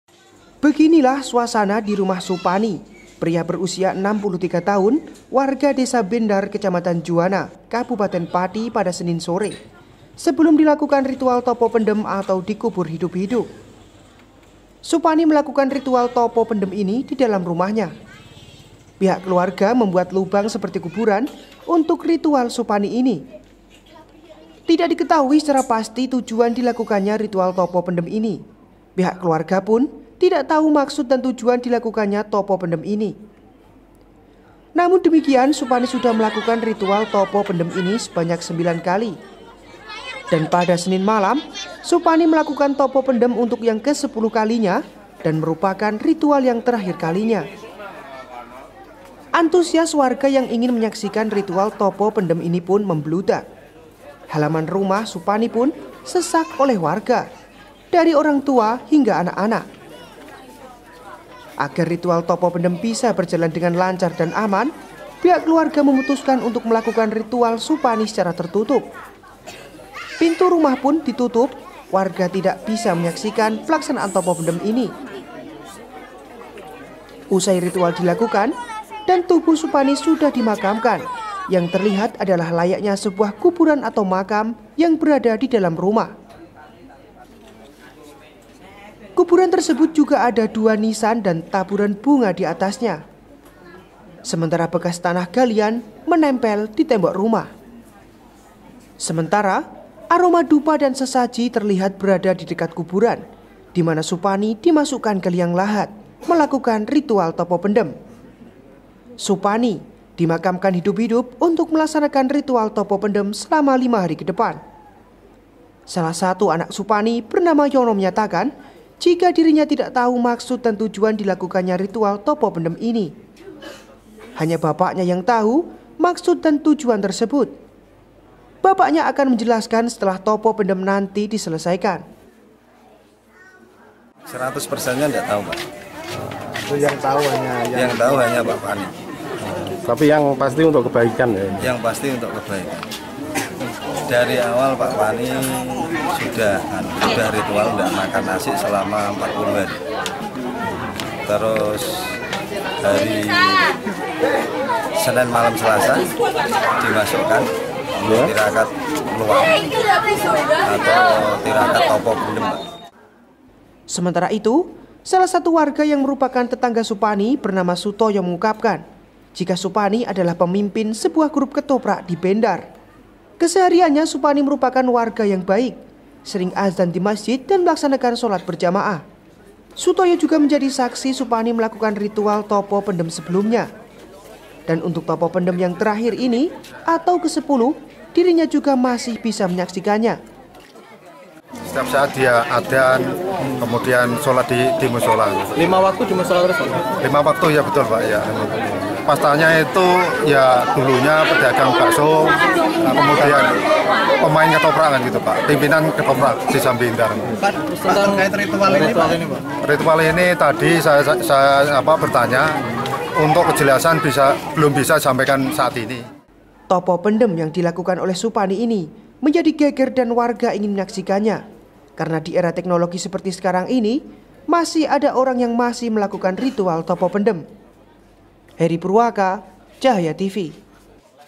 Warga Bendar Juwana Ini, Kembali Jalani Ritual Topo Pendem Yang Ke 10 Kali
Seorang Warga Desa Bendar , Kecamatan Juwana , Kabupaten Pati , Membuat Heboh, Lantaran Melakukan Ritual Topo Pendem , Atau Dikubur Hidup-Hidup . Warga Tersebut Bernama Supani Berumur 63 Tahun , Dan Menurut Keluarga Sudah Melakukan Topo Pendem Ini , Untuk Yang Kesepuluh Kali , Pada Senin Malam  .